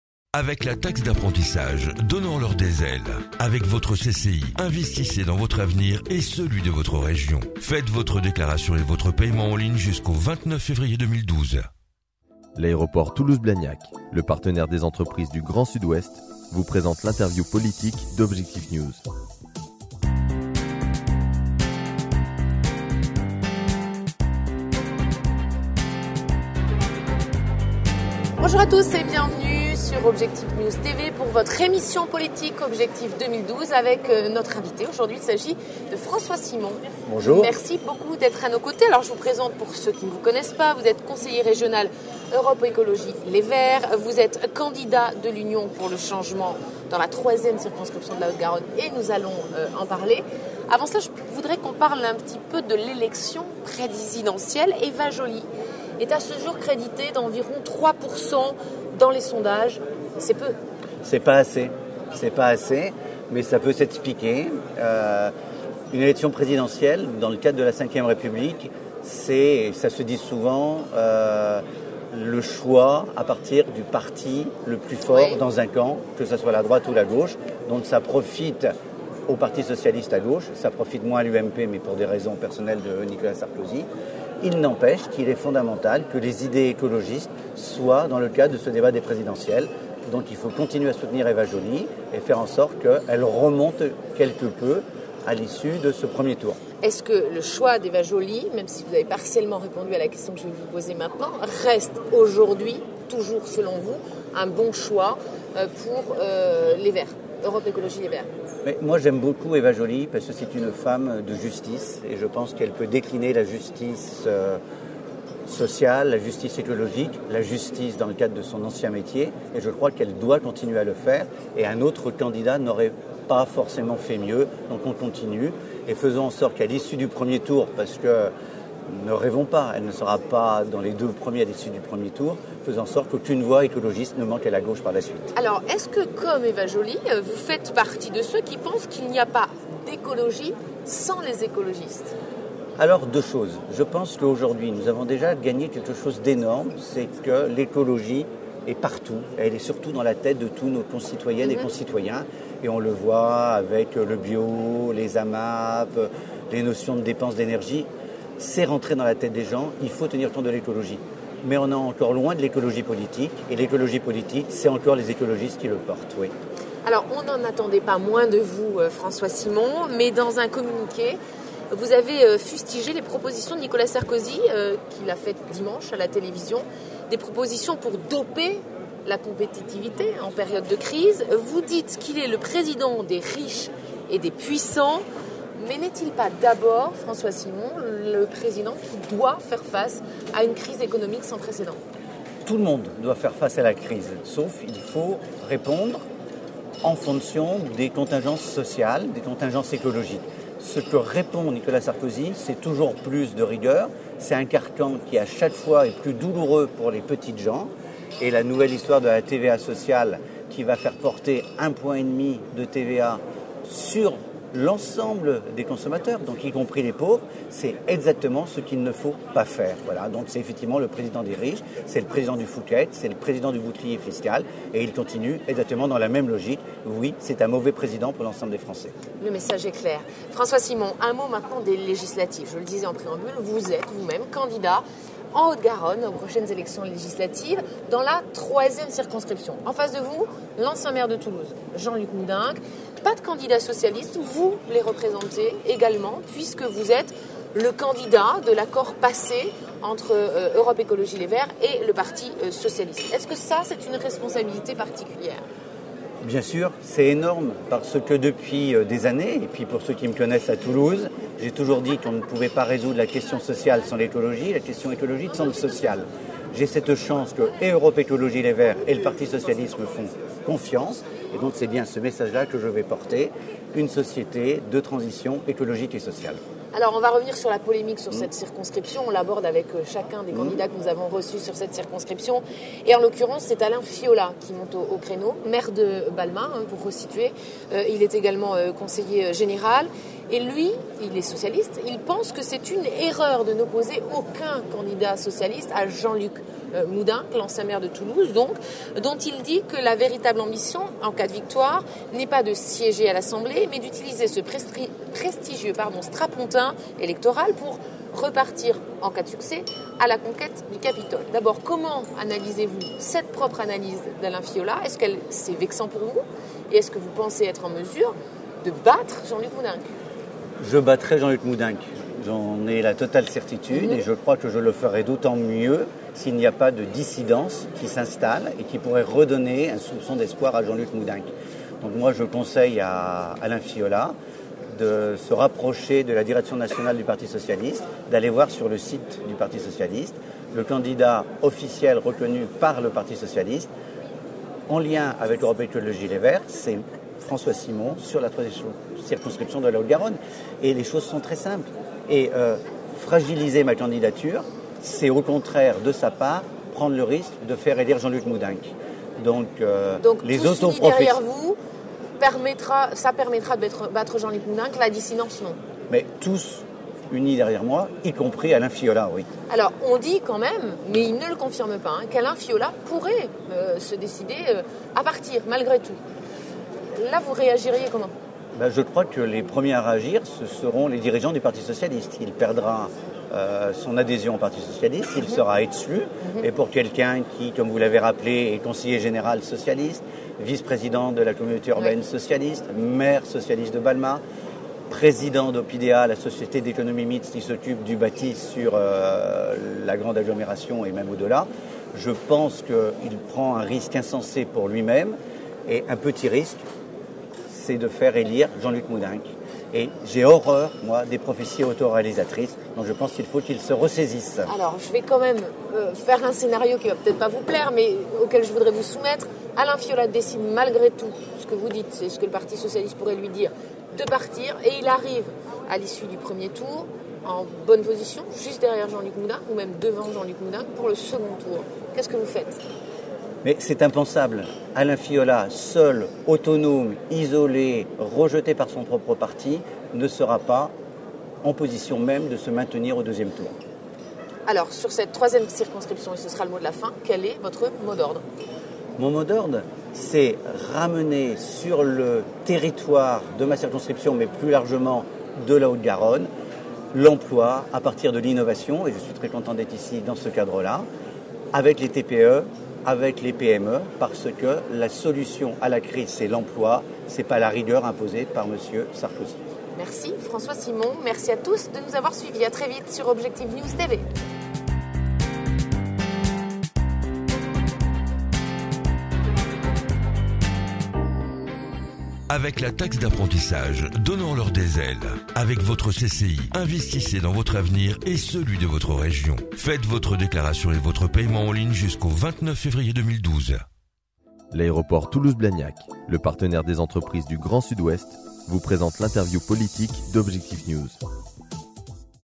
L'interview politique. François Simon, Vice Président du Conseil régional en charge des solidarités
L'écologiste François Simon évoque sa candidature aux législatives dans la troisième circonscription de Toulouse. Il revient également sur le faible taux d'intentions de vote pour Eva Joly aux présidentielles et fustige Nicolas Sarkozy sur ses mesures de relance de la compétitivité.